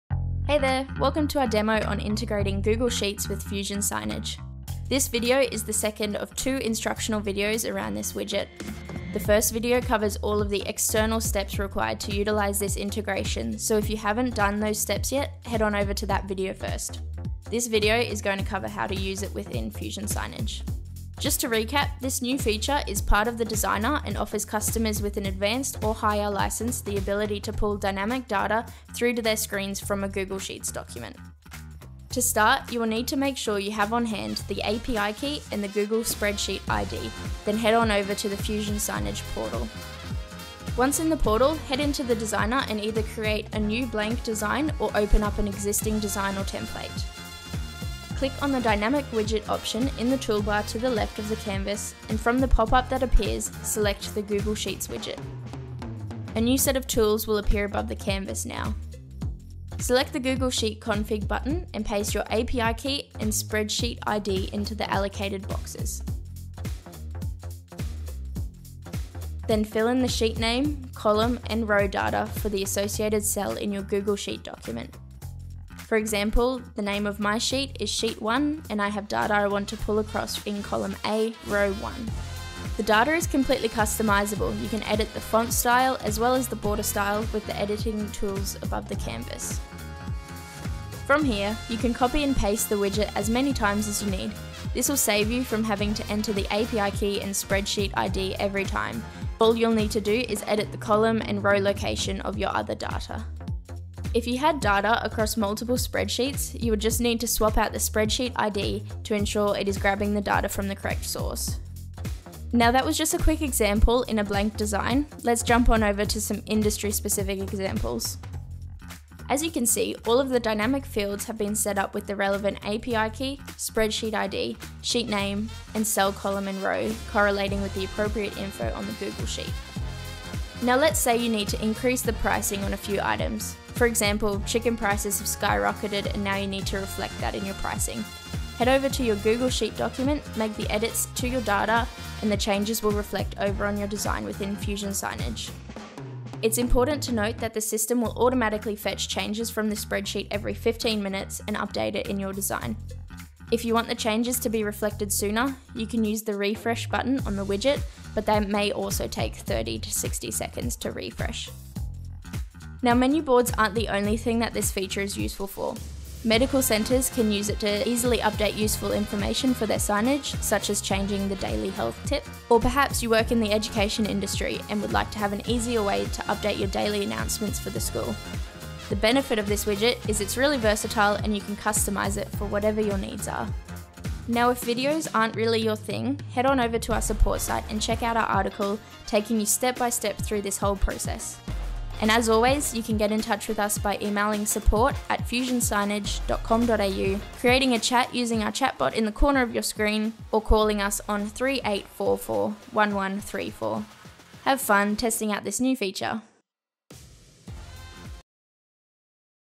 Google Sheets Widget: In Action
Learn how to use the Google Sheets Widget across various industries.